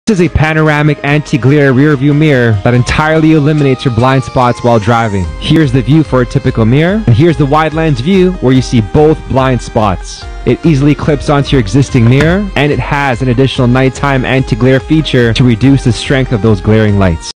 Anti-glare Panoramic Rearview Mirror
Product: 300mm Convex Anti-glare Wide-angle Car Rearview Mirror

Material: Glass+ABS

Type: 30cmX6.5cm

Suitable: Universal for all the cars

Feature:

--The level of surface design is curved for a better view.

--Anti-glare wide-angle mirror.

--High-definition glass makes the image clear and sharp, and has a wide field of vision.

--Easy to install, the angle of the mirror can be adjusted.

--High safety and safer driving.

Noted:

The pictures are for reference only. The actual product shall prevail. Your satisfaction is our most concern.